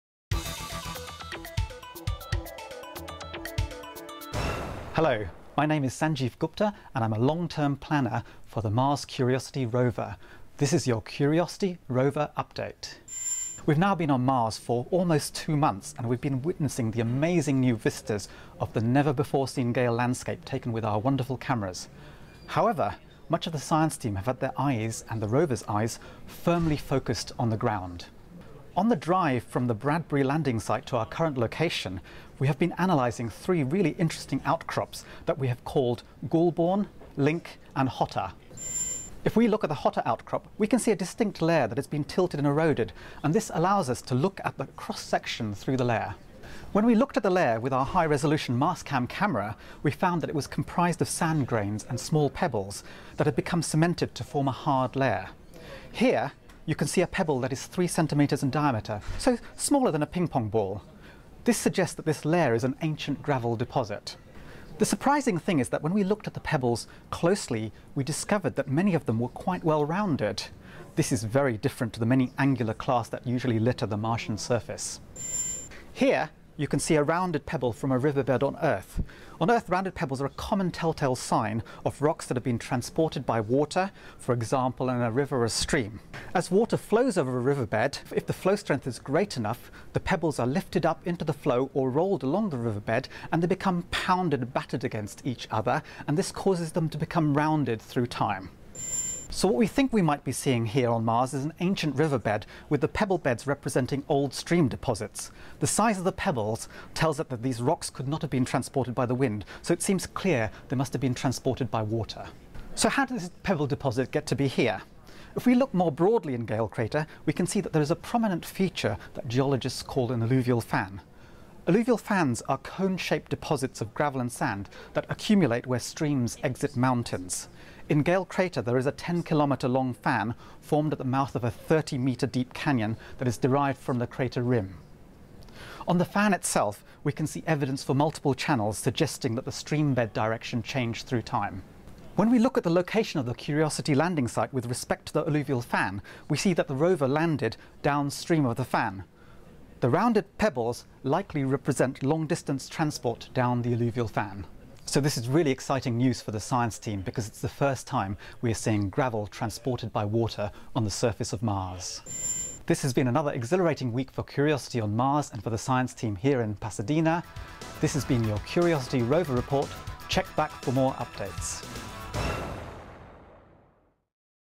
NASA's Mars Curiosity Rover Report #8
A NASA's Mars Curiosity rover team member gives an update on developments and status of the planetary exploration mission. The Mars Science Laboratory spacecraft delivered Curiosity to its target area on Mars at 1:31:45 a.m. EDT on Aug. 6, which includes the 13.8 minutes needed for confirmation of the touchdown to be radioed to Earth at the speed of light. The rover will conduct a nearly two-year prime mission to investigate whether the Gale Crater region of Mars ever offered conditions favorable for microbial life.

Curiosity carries 10 science instruments with a total mass 15 times as large as the science payloads on NASA's Mars rovers Spirit and Opportunity. Some of the tools, such as a laser-firing instrument for checking rocks' elemental composition from a distance, are the first of their kind on Mars. Curiosity will use a drill and scoop, which are located at the end of its robotic arm, to gather soil and powdered samples of rock interiors, then sieve and parcel out these samples into the rover's analytical laboratory instruments.